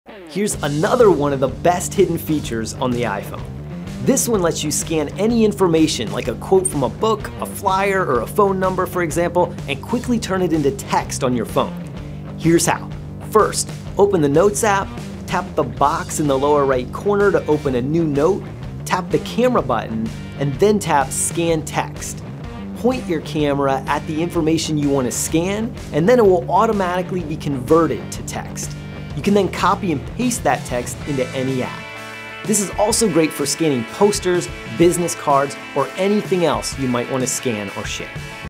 Use your iPhone to instantly scan text
Your iPhone can automatically scan flyers, business cards, and more to turn it into text to save or share.